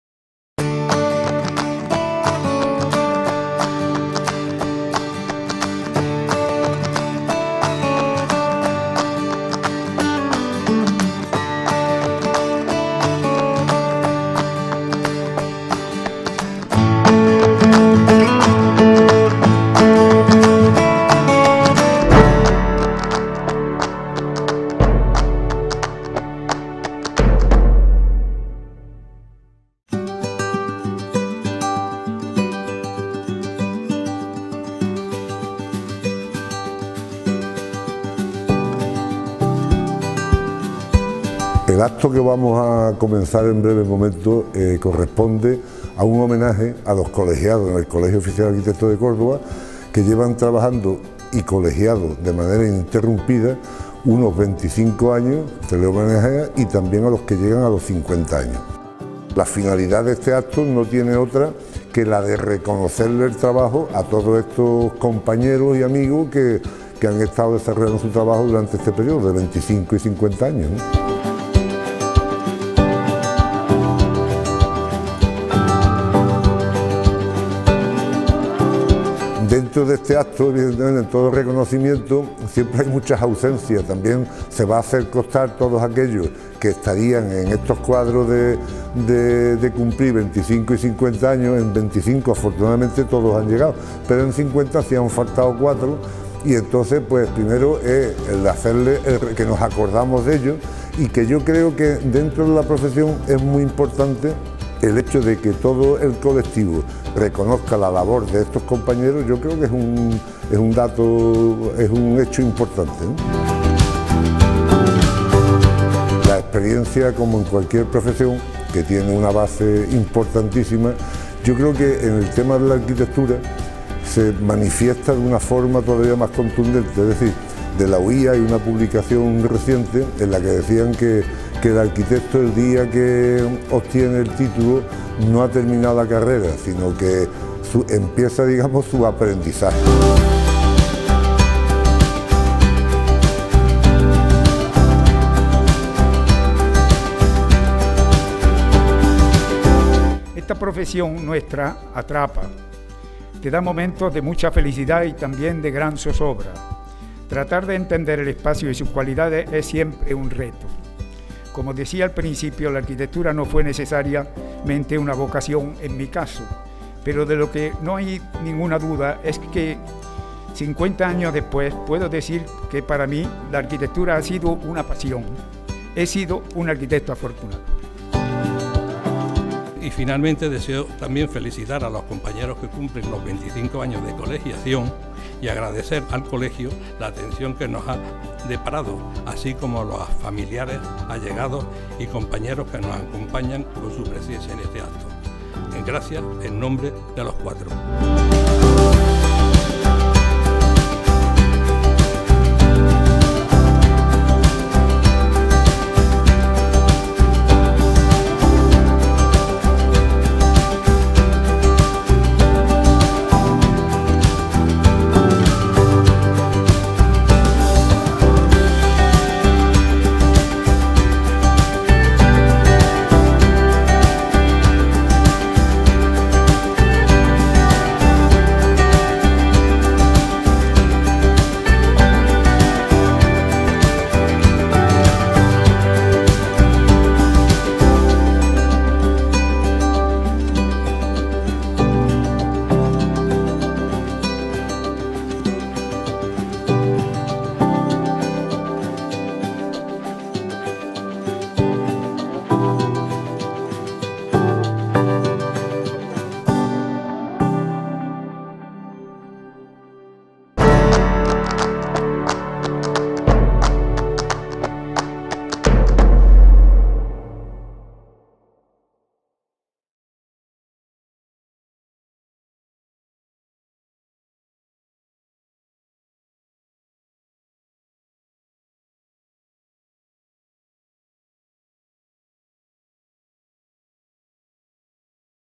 Homenaje a los 25 y 50 años de colegiación, 2021
El Colegio Oficial de Arquitectos de Córdoba homenajea a los arquitectos y arquitectas que cumplen 25 y 50 años de colegiación en los años 2019 y 2020